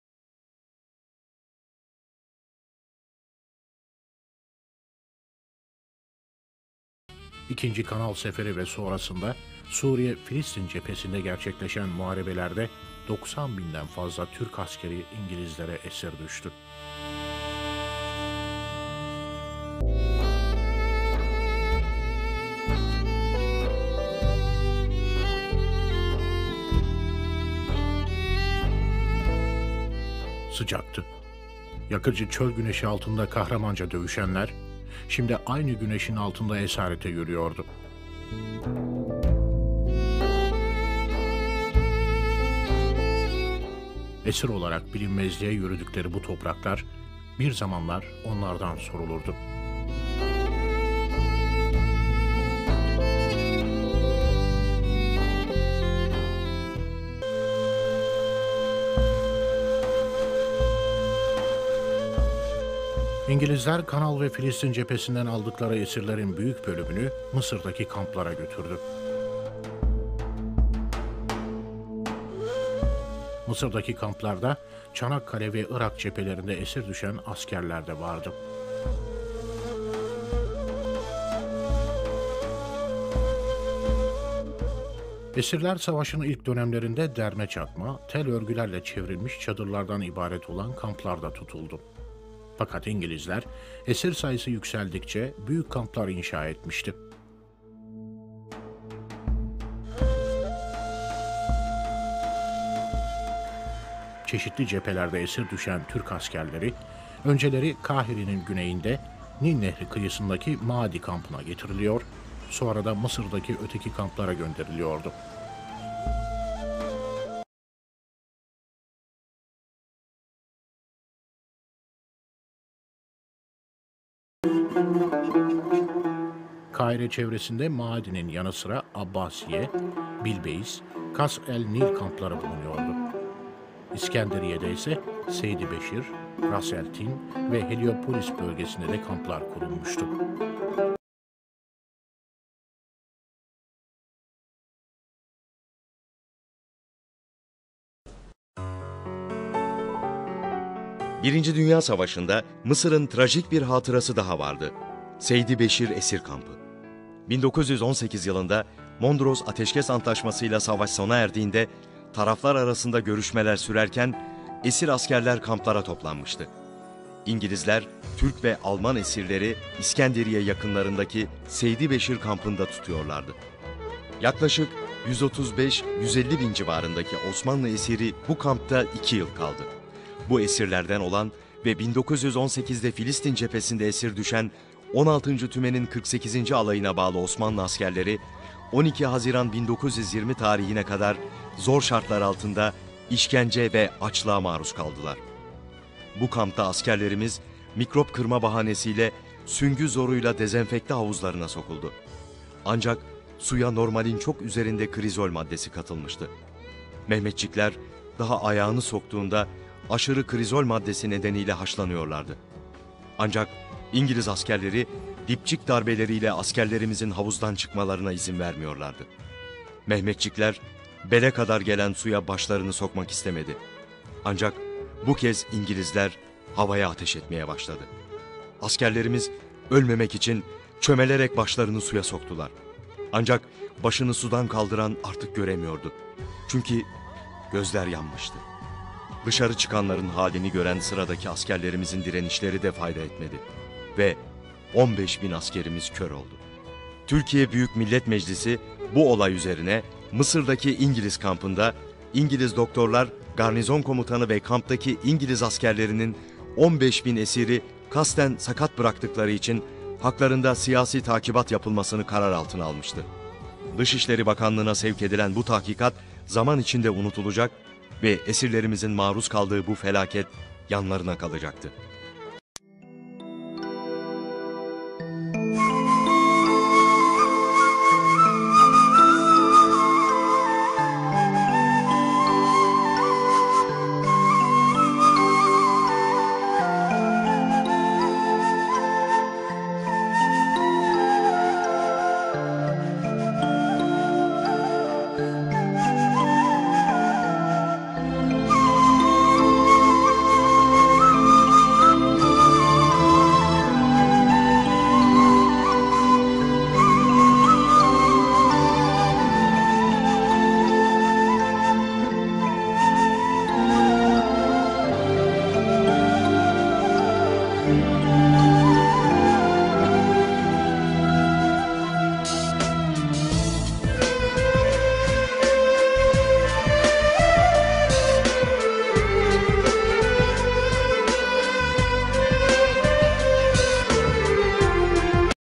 ESİRLERİMİZİN SEYDİBEŞİR ESİR KAMPI' NDA KÖR EDİLMESİ
1. DÜNYA SAVAŞINDA MISIR' DAKİ TRAJİK HATIRA; SEYDİBEŞİR ESİR KAMPI